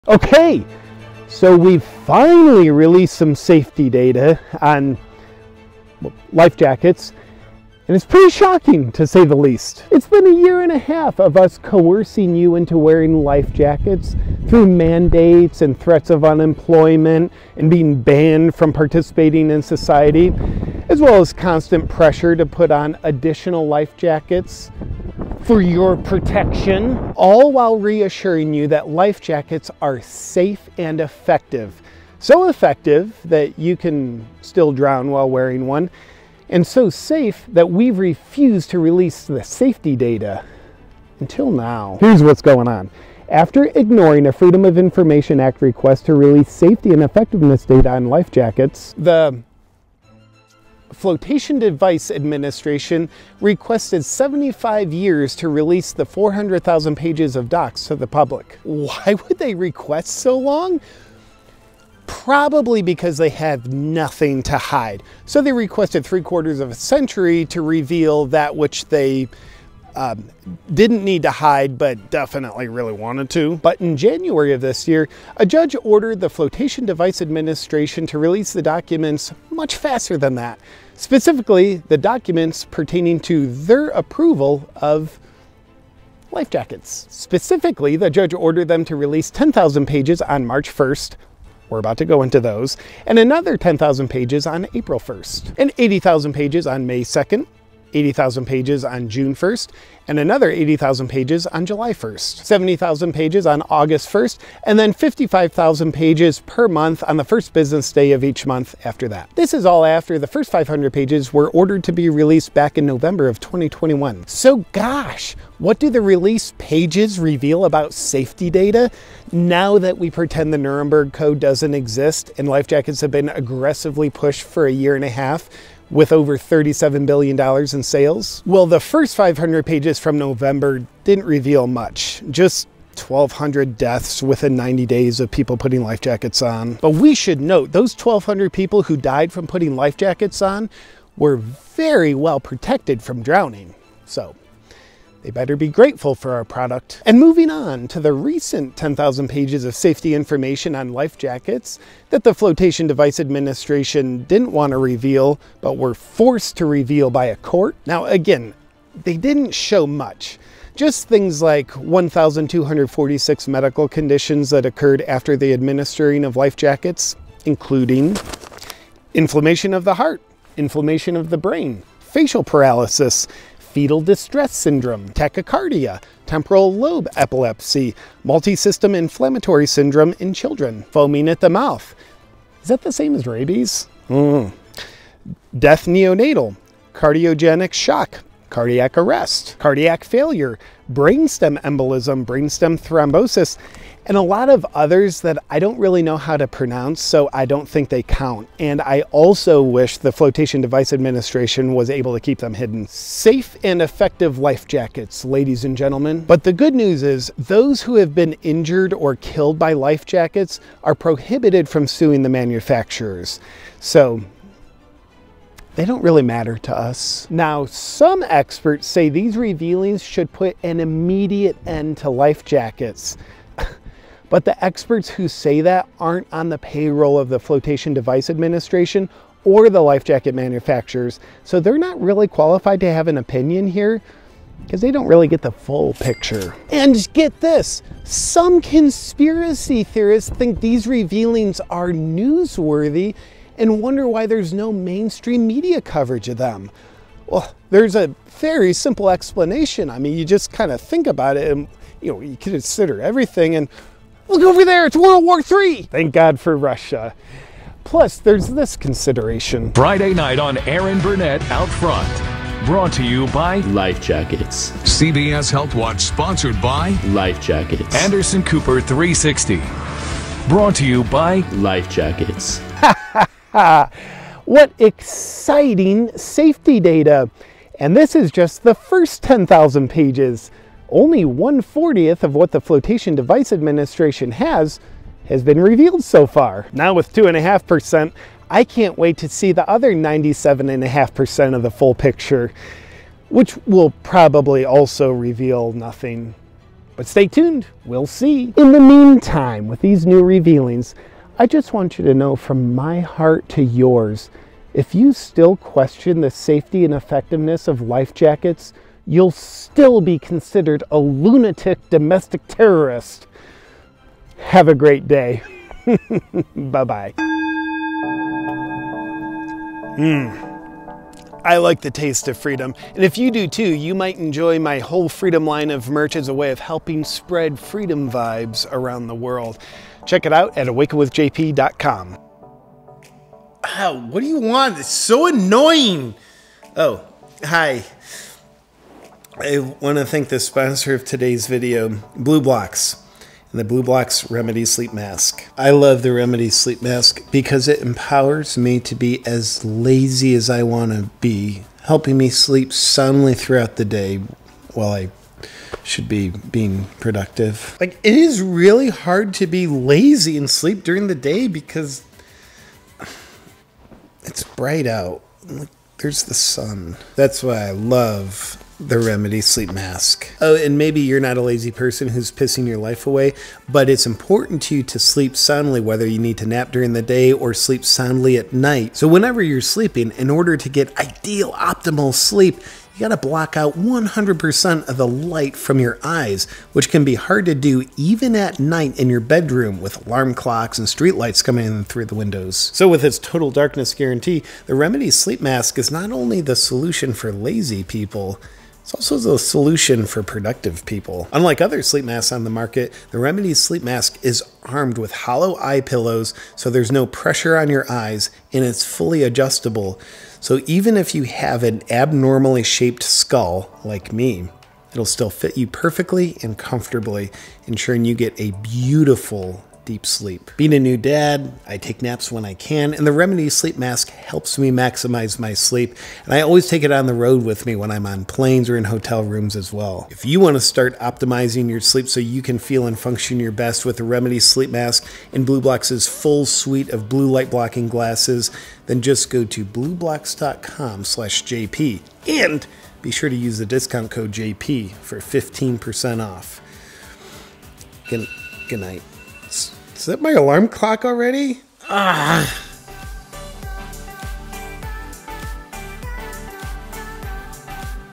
BOMBSHELL Safety Data Released on “Life Jackets”
Take a stand against censorship.

A new bombshell report on safety data for life jackets has been released by the Flotation Device Administration. Watch this video for everything you need to know.